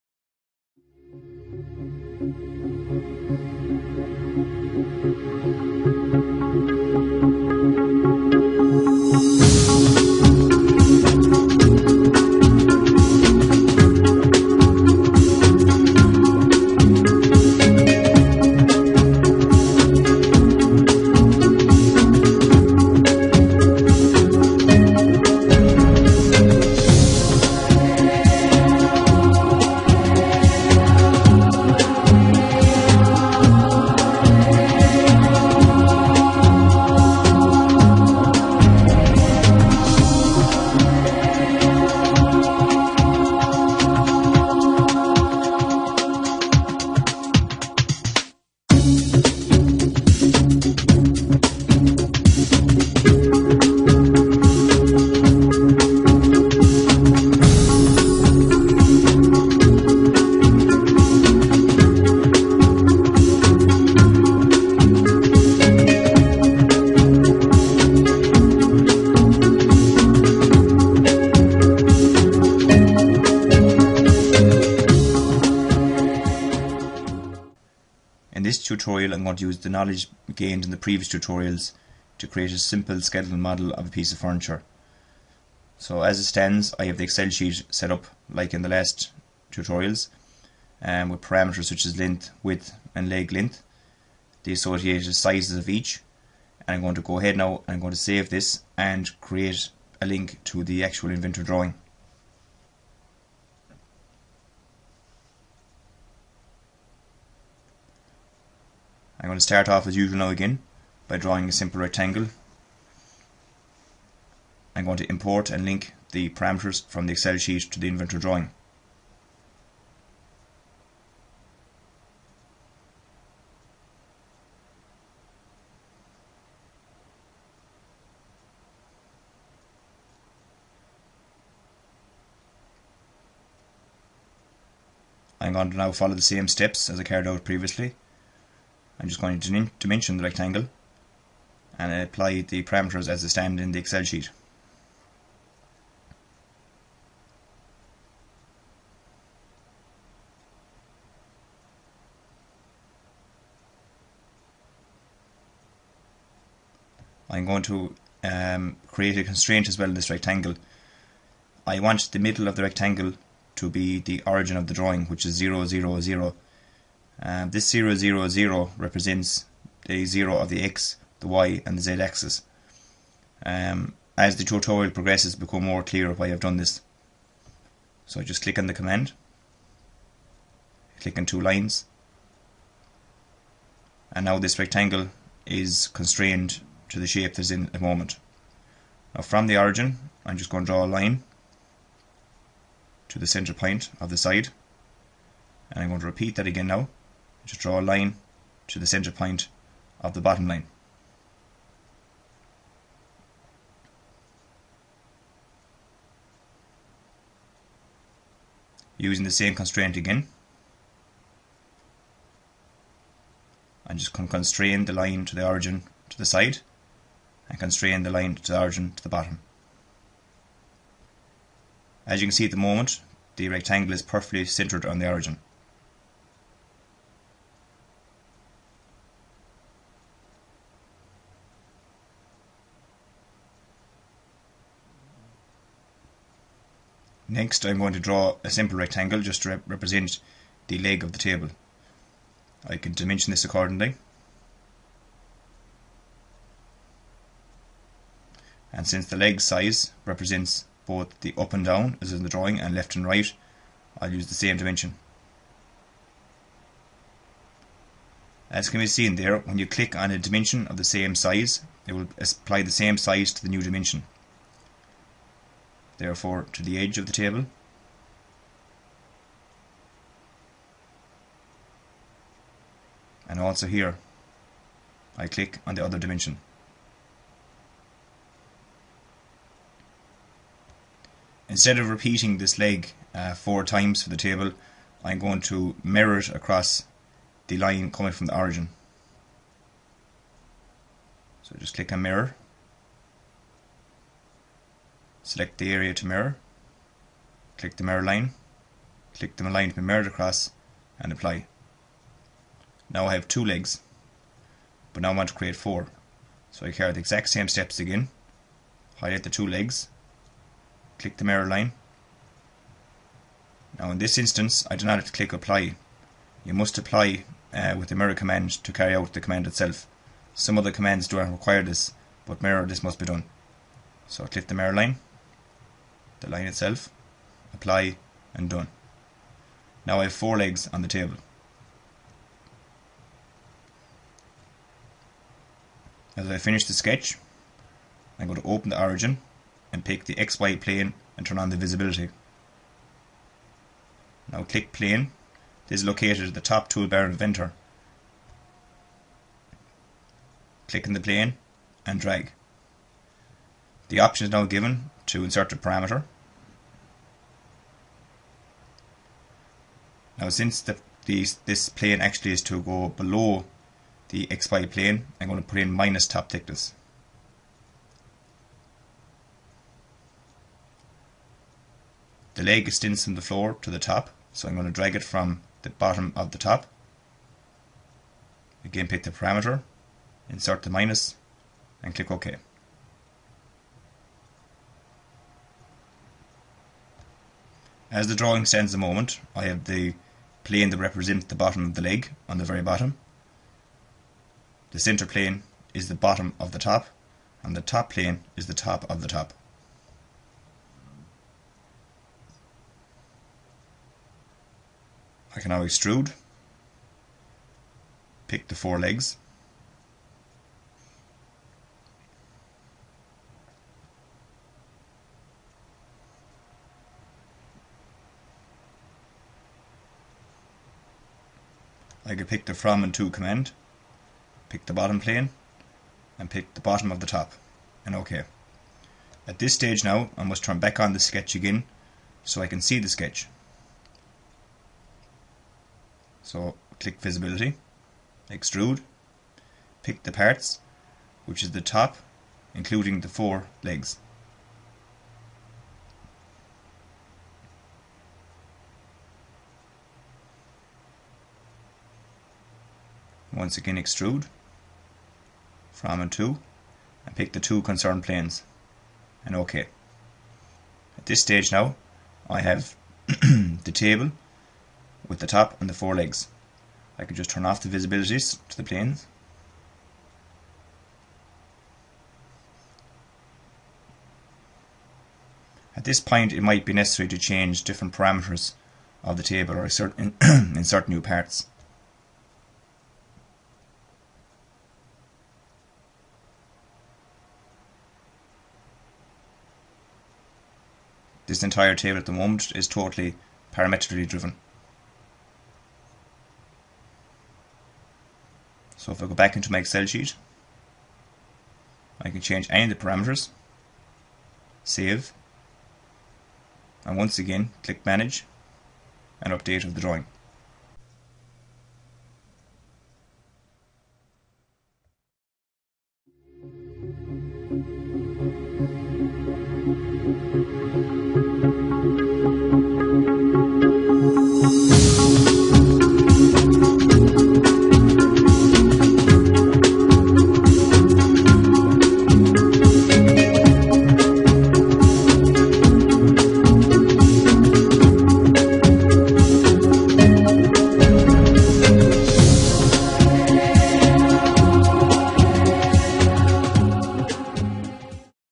Tutorial 4 Part 1: Using Tutorials 1-3 To Produce Simple Furniture
To reflect upon, and integrate the knowledge gained to date of Inventor in previous tutorials.  This is demonstrated in this tutorial by the production of simple furniture.